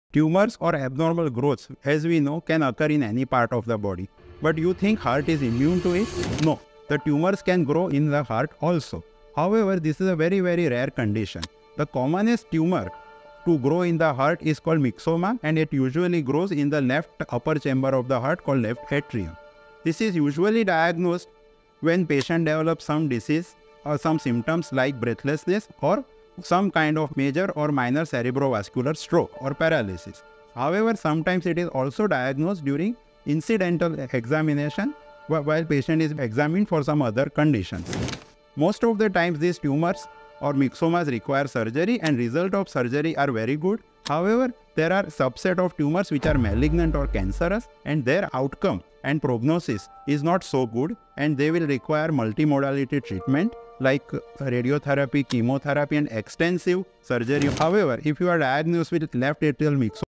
Do you think heart is immune to Tumors?
Check out Dr. Apurv Vaidya Cardiothoracic Surgeon @aadicurahospitals to share his views on this topic.
Visit @aadicurahospitals,
Jetalpur Rd, behind Gujarat Kidney Hospital, Anand Nagar, Haripura, Vadodara, Gujarat 390020📍